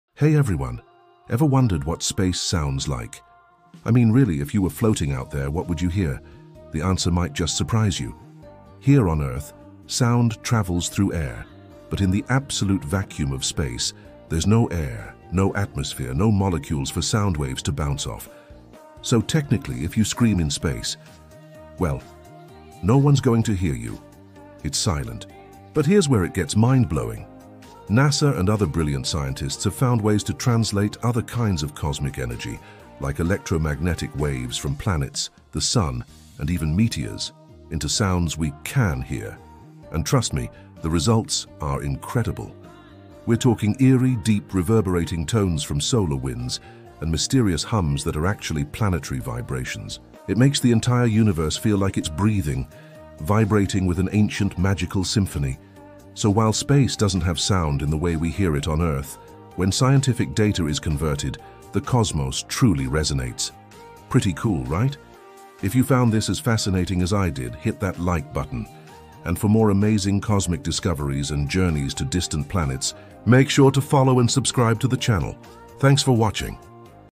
Cosmic Sounds: How Space Really “Speaks”space
What does space sound like? While outer space is mostly a vacuum, which means sound waves cannot travel as they do on Earth, scientists can convert electromagnetic vibrations and plasma waves into sound. From the eerie hum of planets to the crackling of solar winds, space has a cosmic symphony that reveals the hidden rhythms of the universe. Explore the science behind these sounds and how they help us understand cosmic phenomena.